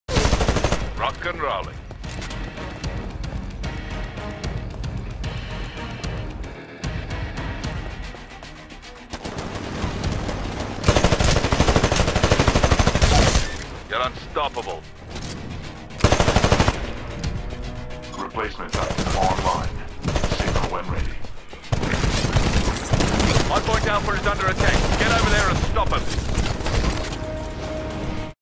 Kill Streak pt 2
Second part to my 7 pilot kill streak